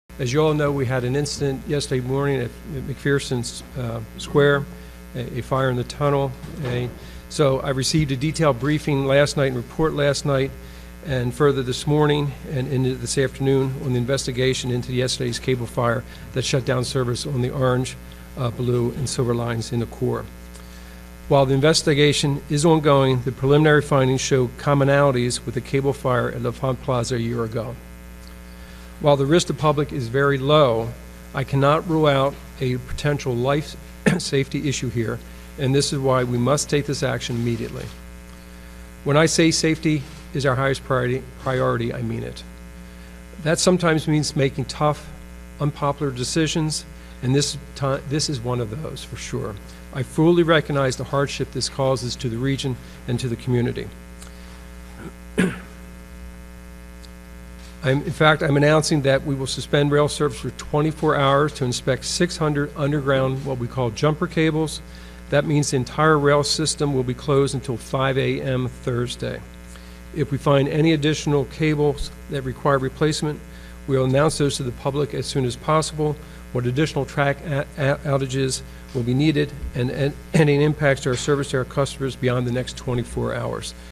Entire DC Metro To Shut Down For Inspections
March 15, 2016 - The rail system that serves the nation's capital and its Virginia and Maryland suburbs will shut down for a full day after a fire near one of the system's tunnels, the system's head announced. (March 15)